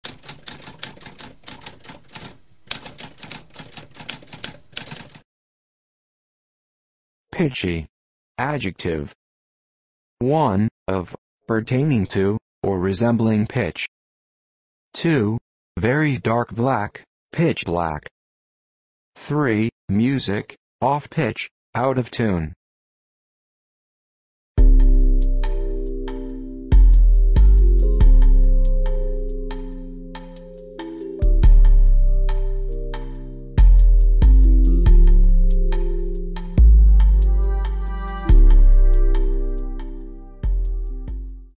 What does pitchy mean?
A spoken definition of pitchy.

Intro Sound:
Typewriter - Tamskp